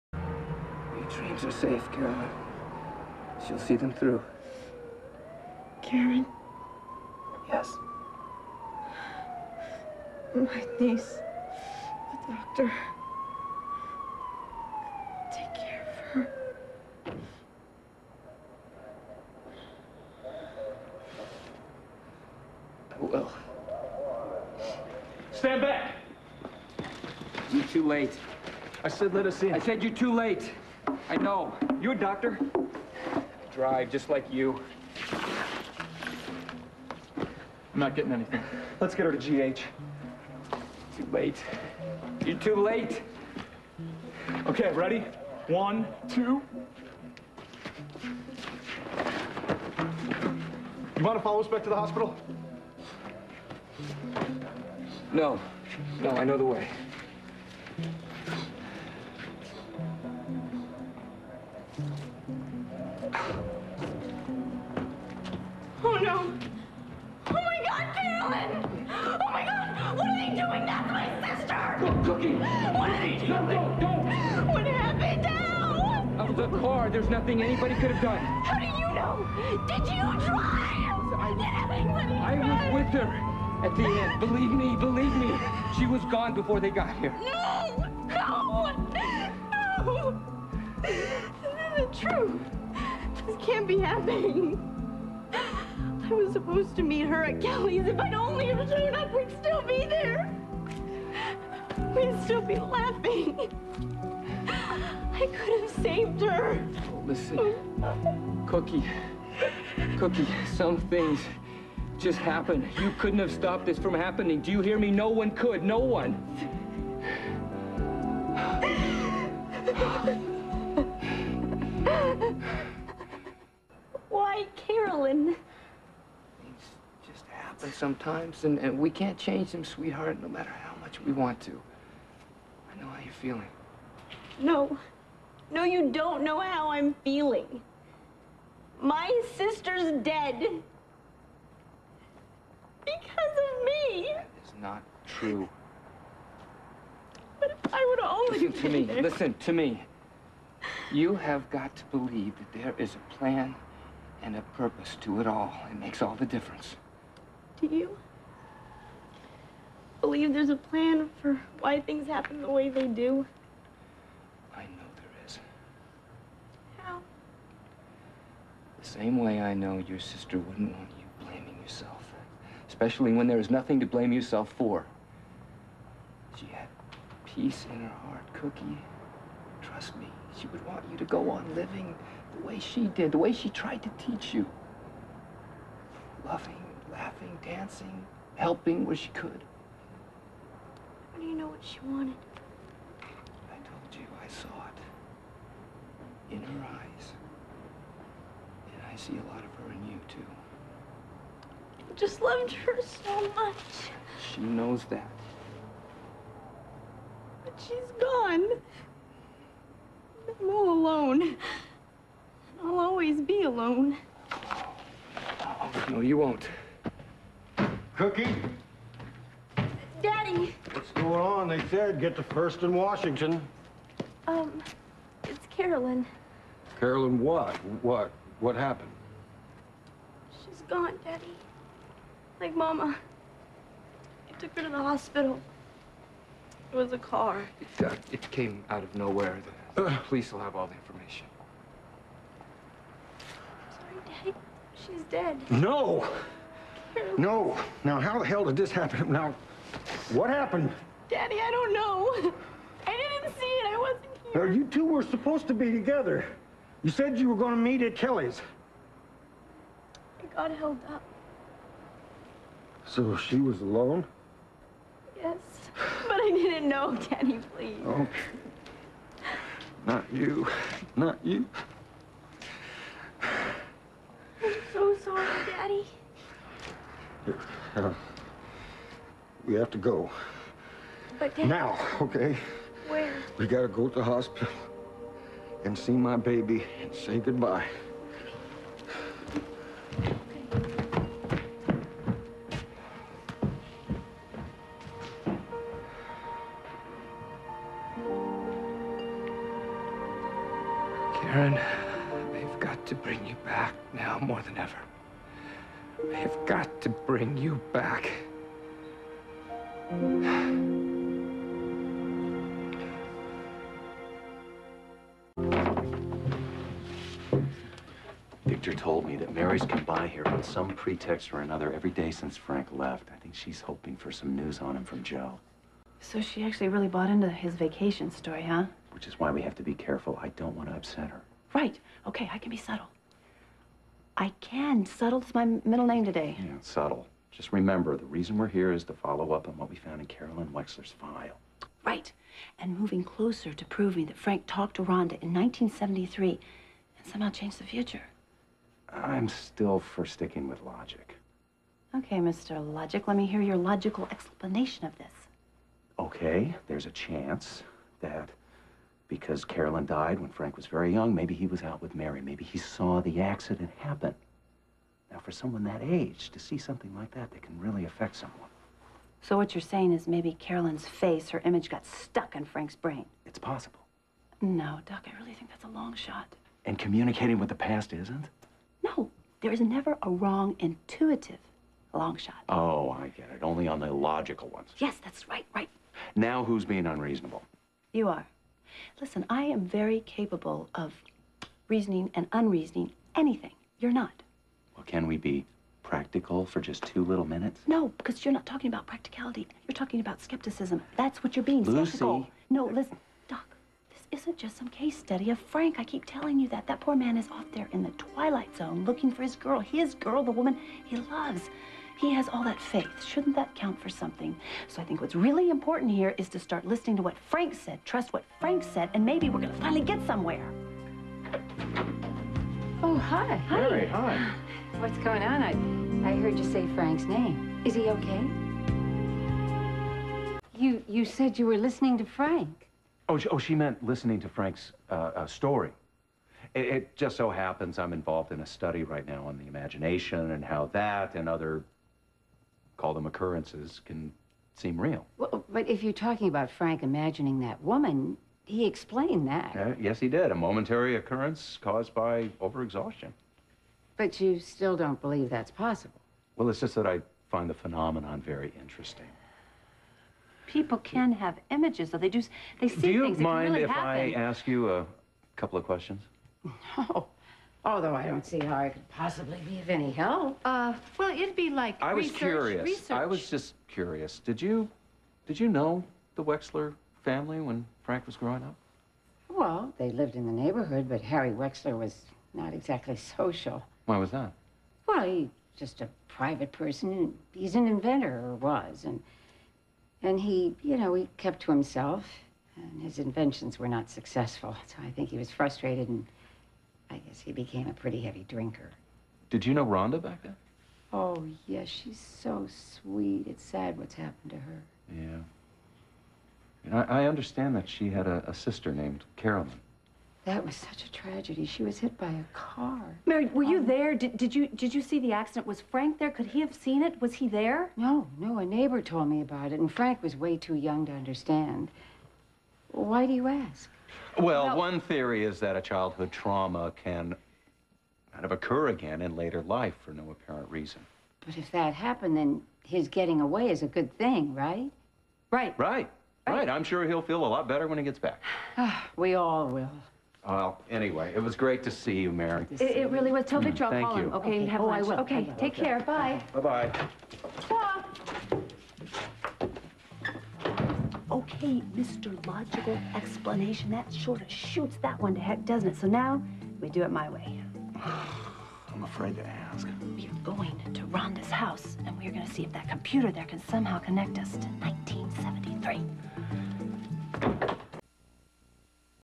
Search thru time- 32- TIAB (3of 4)
then and now (the alternate universe of the present) no matter how many times I see the scene between Kevin & Lucy, I crack up every time I see Kevin's expression/stance at the end. :)